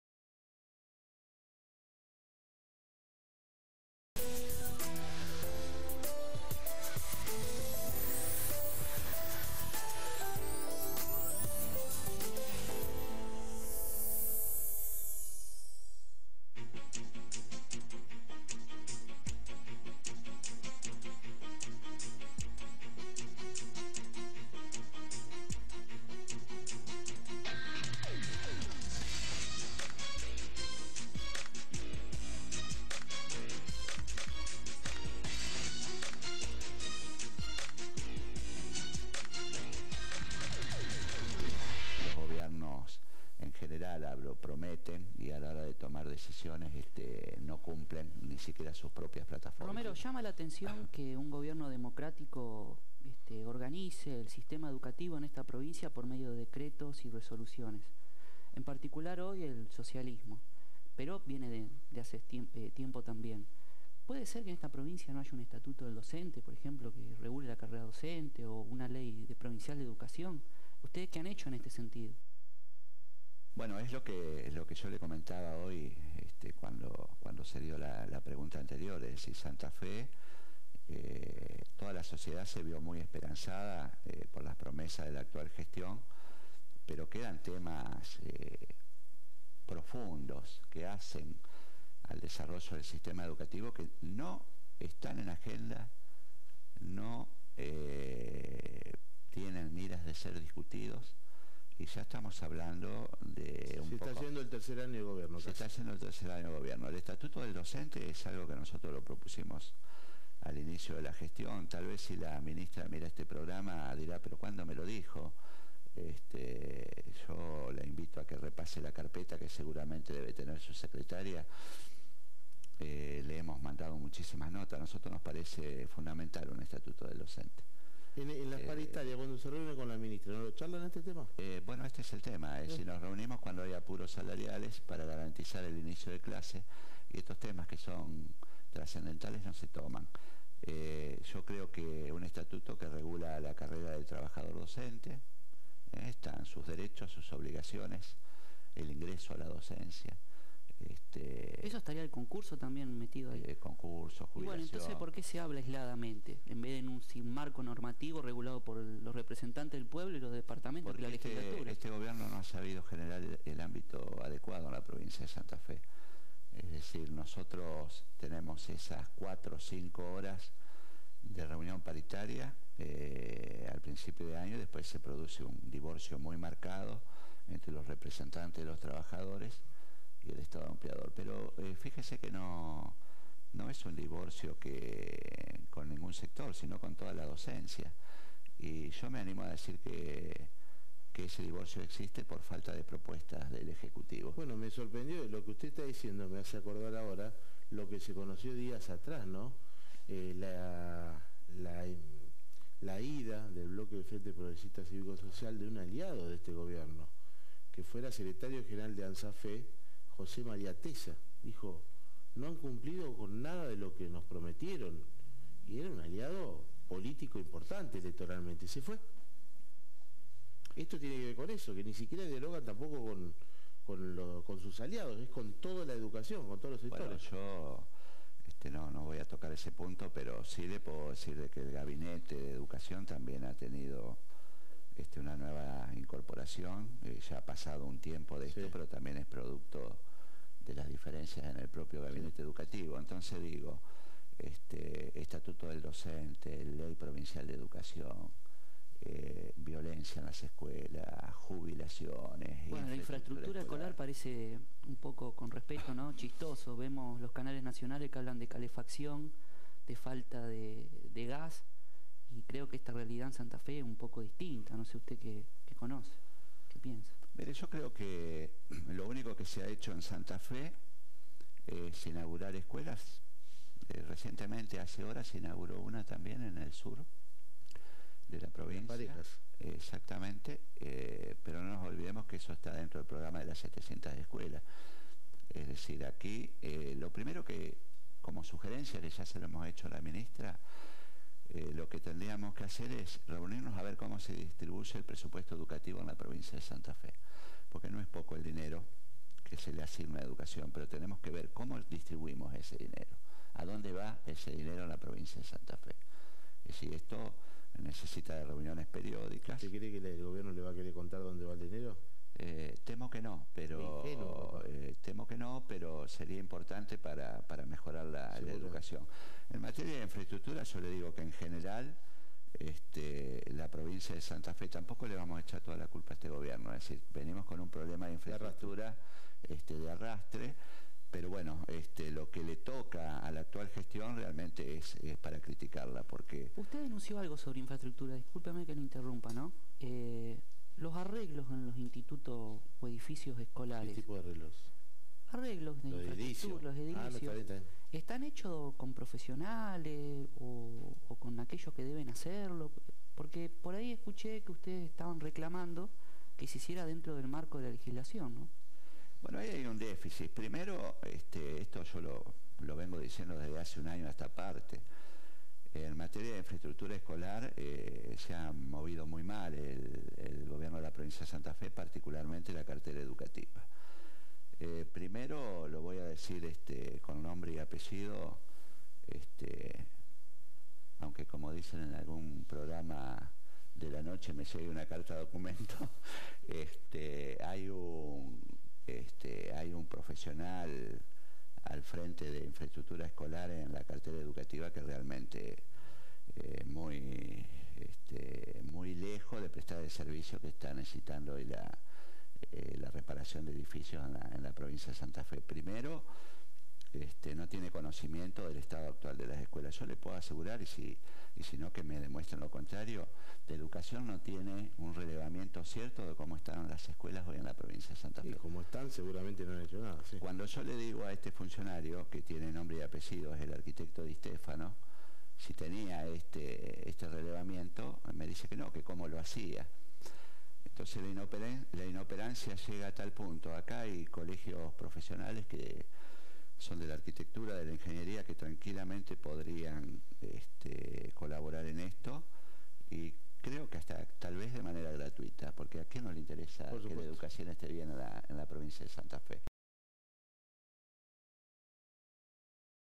Santa Fe: SERGIO ROMERO (UDA) en NUESTRAS VOCES (3 Parte)
(3 PARTE) SERGIO ROMERO, HABLA LA SITUACIÓN EN SANTA FE.